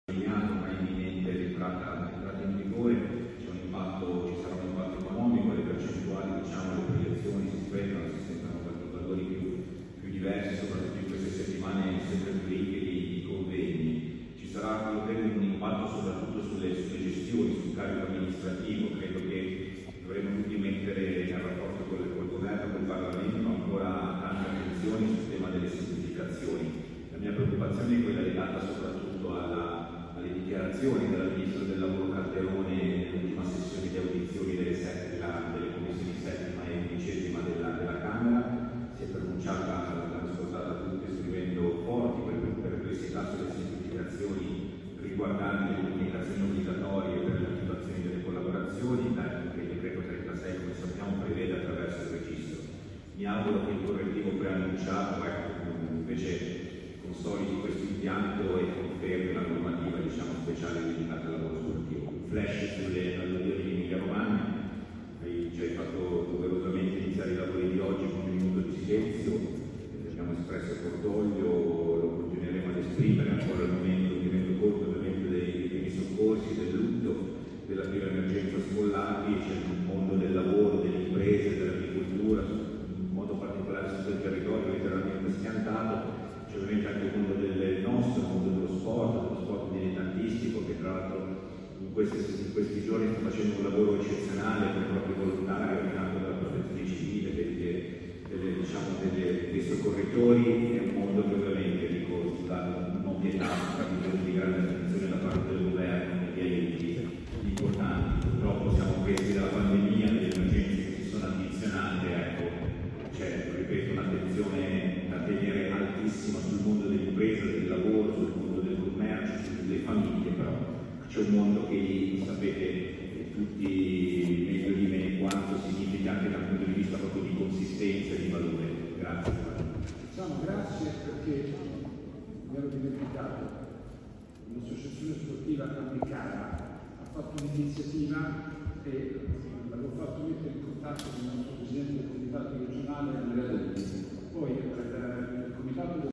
L'intervento di Tiziano Pesce , presidente nazionale UISP al 296esimo Consiglio Nazionale Coni.
Tiziano Pesce , presidente nazionale UISP Aps  è intervenuto lunedì 22 maggio, nel salone d’Onore del Foro Italico, a Roma, nel corso del 296esimo Consiglio Nazionale Coni.